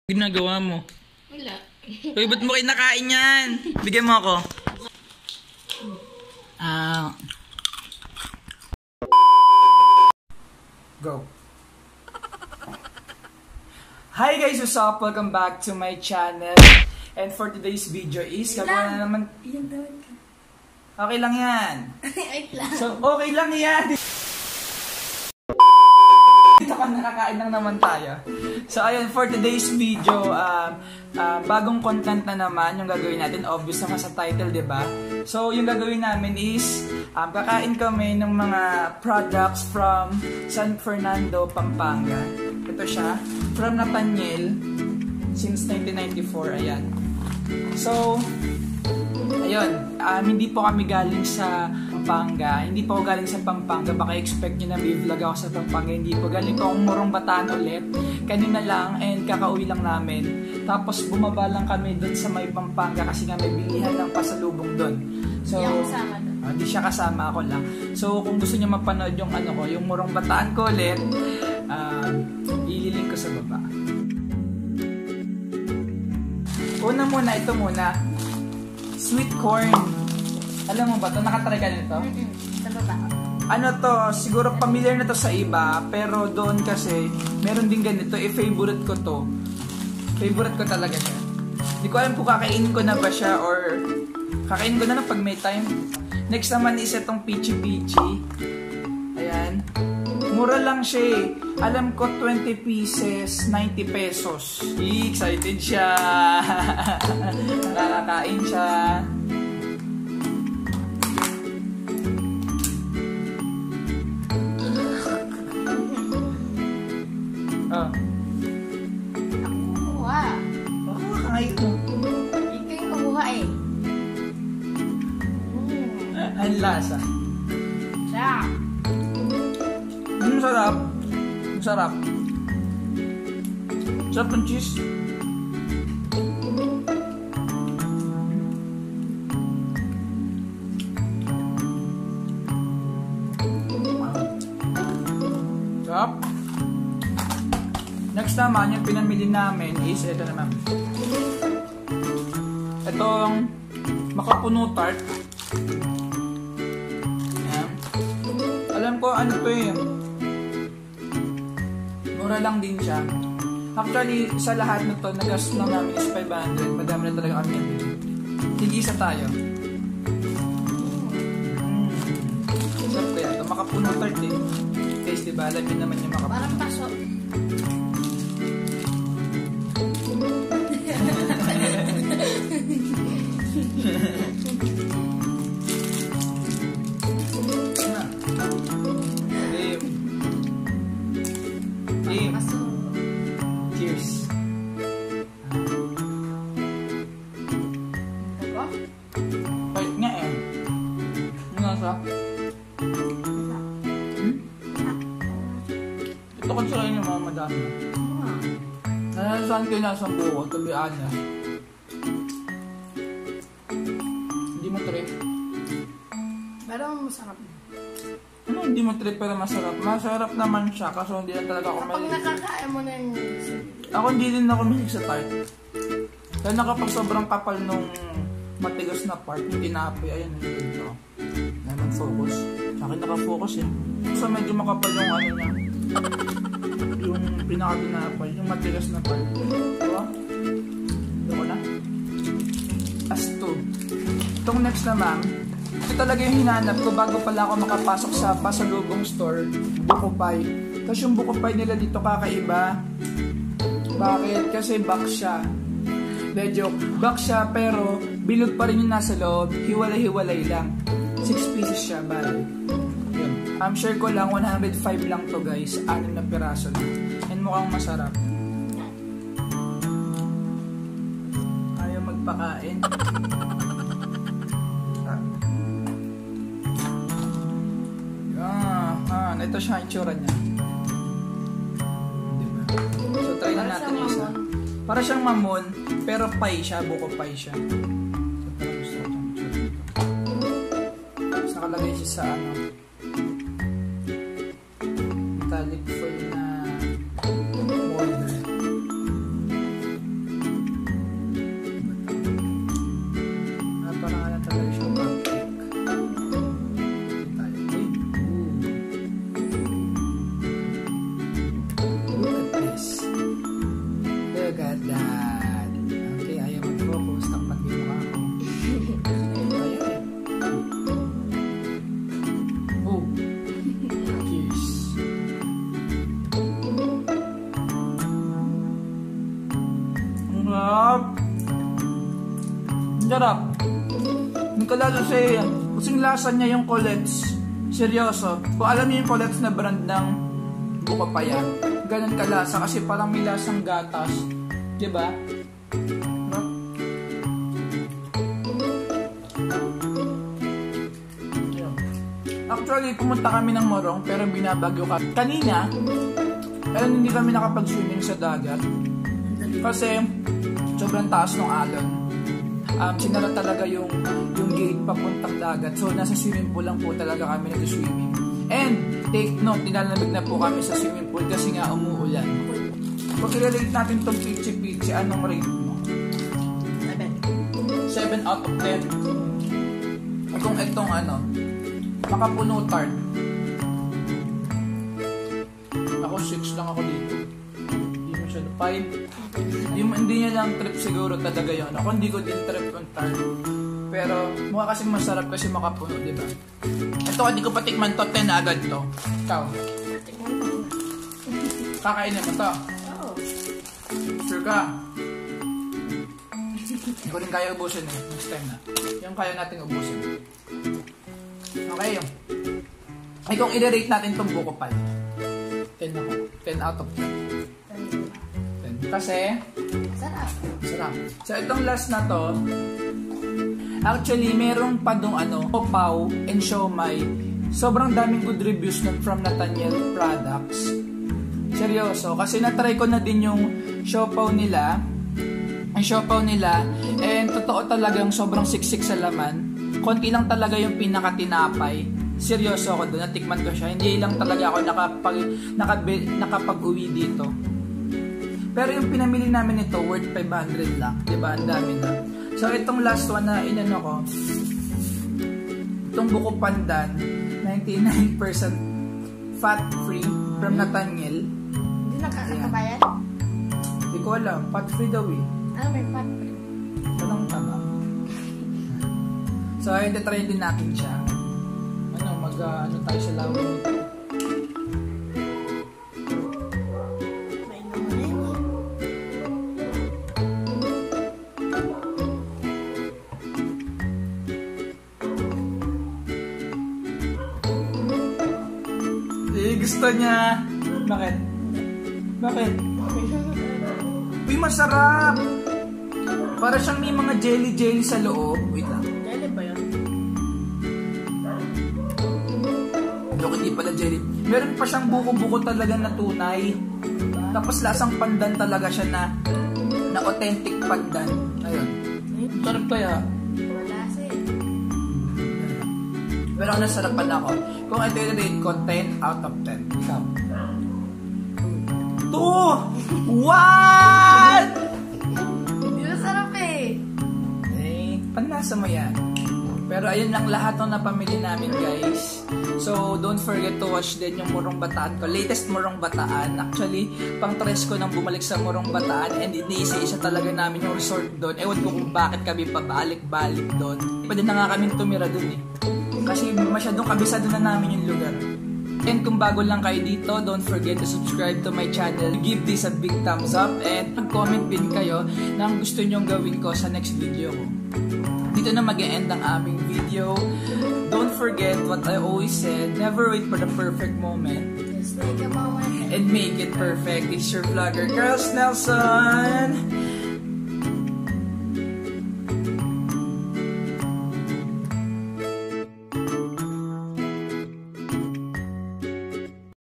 Trying PAMPANGA's BEST PASALUBONG!! (MUKBANG)
I just want to share you and recommend all delicacy I bought, SOBRANG MURA at SARAP NYA :---)